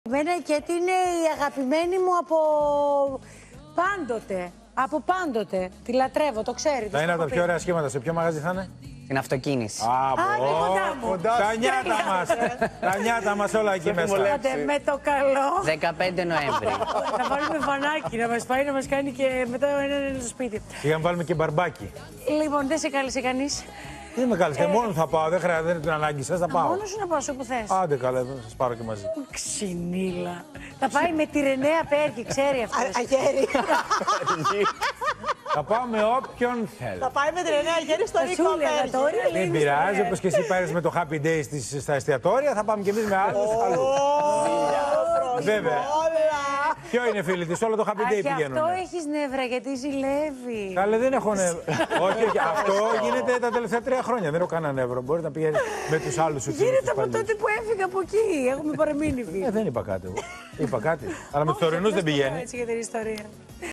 Ελένη Τσολάκη και Λάμπρος Κωνσταντάρας «αλληλοκαρφώνονται» για την Καίτη Γαρμπή | OPEN TV
Γίνετε μέλος στο κανάλι του OPEN για να αποκτήσετε πρόσβαση σε προνόμια:
Και μιας και η Καίτη Γαρμπή θα είναι στην Αυτοκίνηση φέτος, η Ελένη Τσολάκη δεν προτίμησε τον Λάμπρο Κωνσταντάρα για να πάει να την ακούσει. Στην αντεπίθεση πέρασε ο Λάμπρος Κωνσταντάρας.